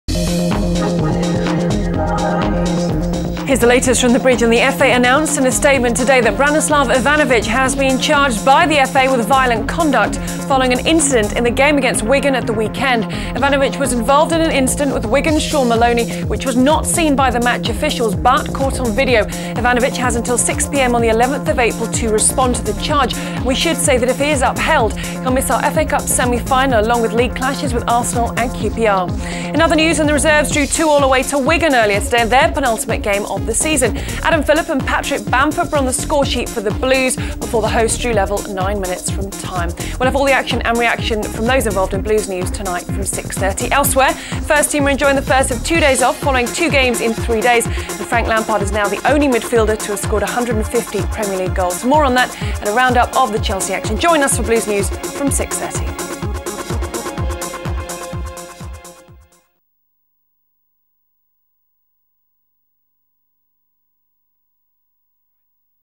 Chelsea FC Blues News Headlines 5pm
Gigi Salmon brings you the latest news from Stamford Bridge